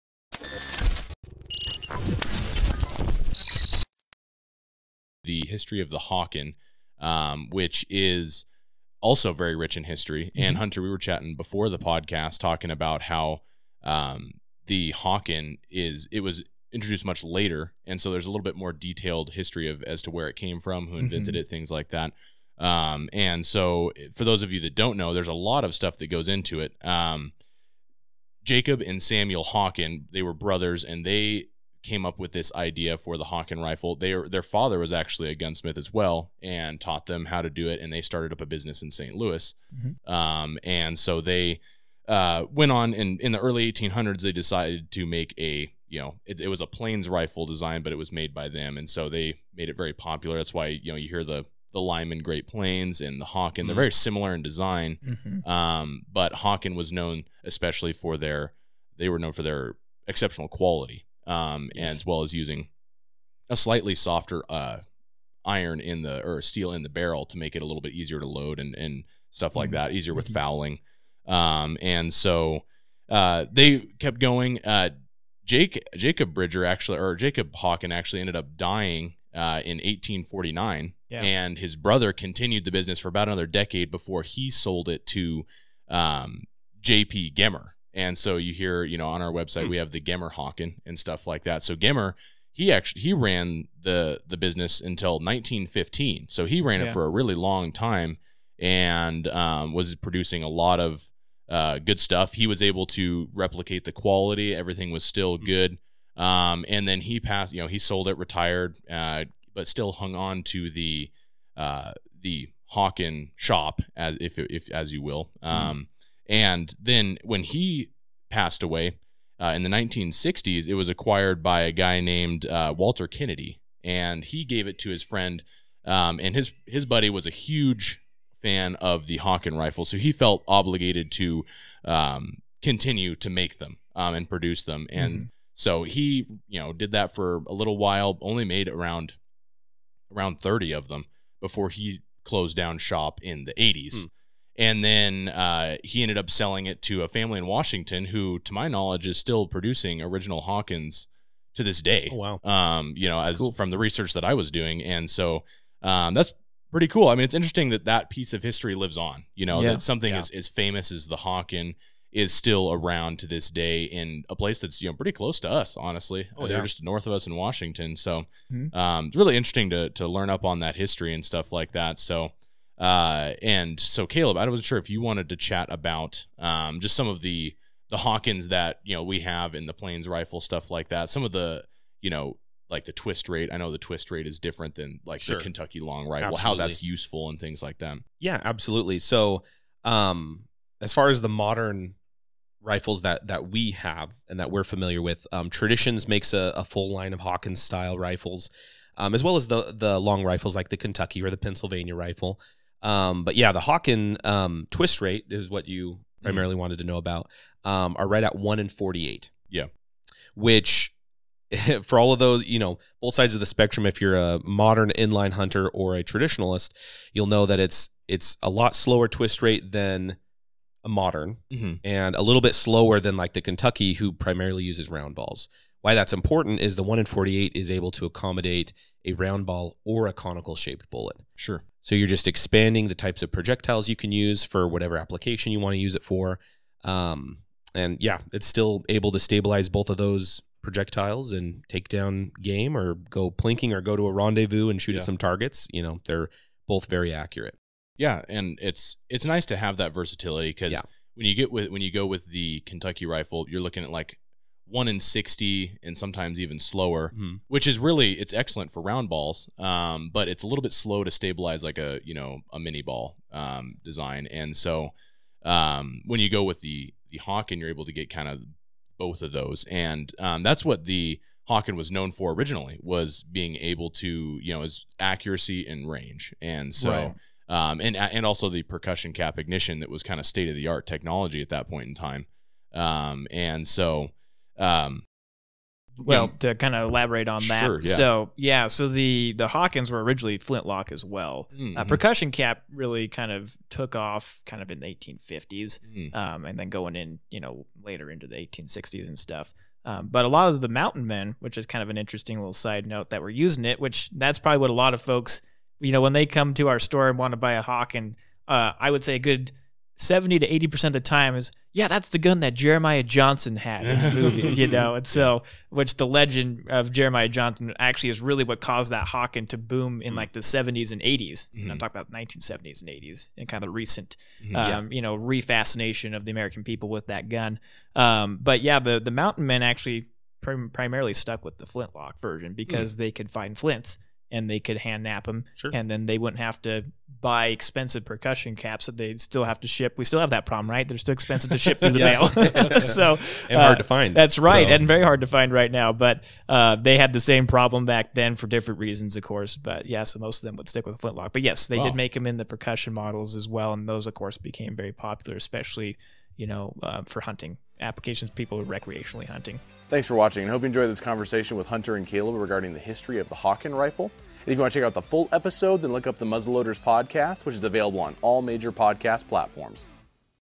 History of the Hawken Rifle - Muzzle-Loaders.com
This is a breif history of the famous Hawken Muzzleloader Rifle. The Hawken Rifle has one of the most interesting backgrounds of any rifle in history! We hope that you enjoy this excerpt from The Muzzle-Loaders Podcast as we discuss the storied past of this amazing muzzleloader.